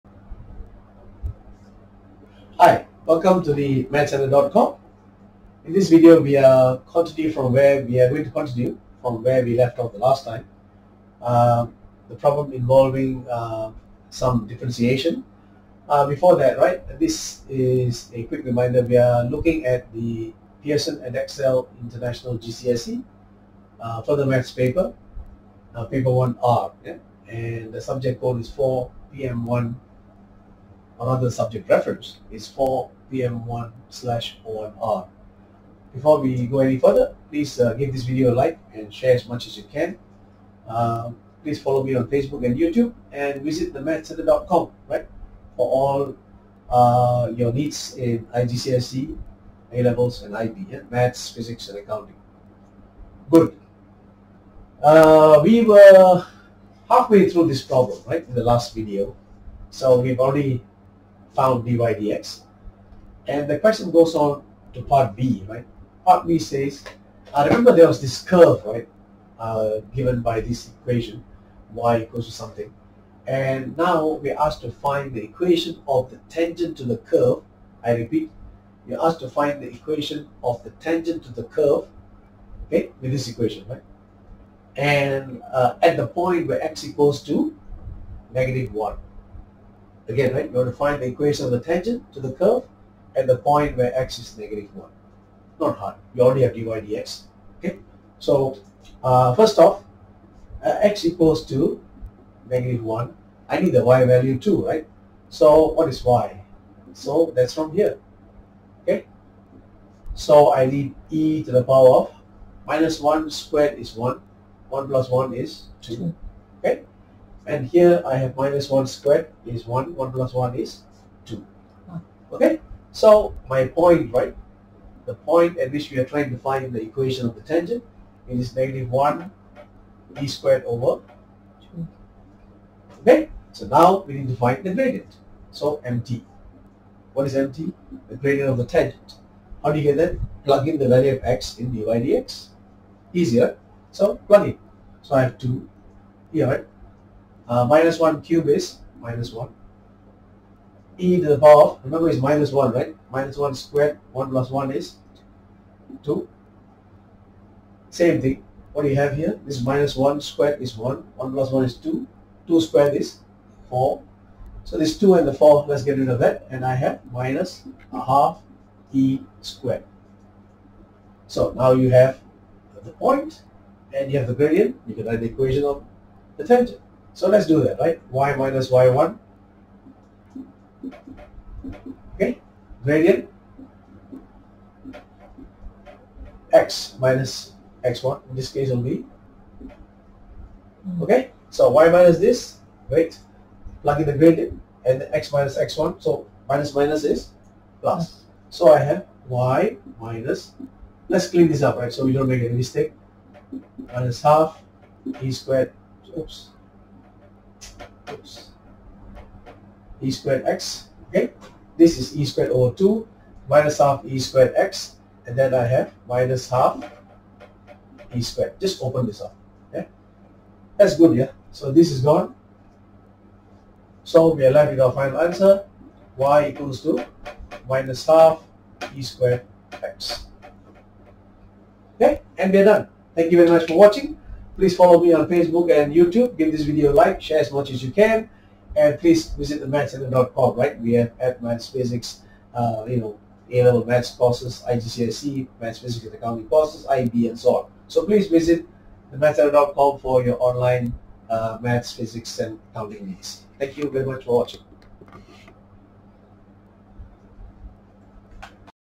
Edexcel IGCSE 4PM1/01R Jan 2023 Exam Solutions Q6b
Edexcel IGCSE Further Pure Maths (4PM1/01R) Jan 2023 Exam Question 6 part b Fully Worked Solutions Video.

Follow us to get notification of our next video.

Also check out our FREE IGCSE Add Maths (0606) Year 10 Special Intro Course !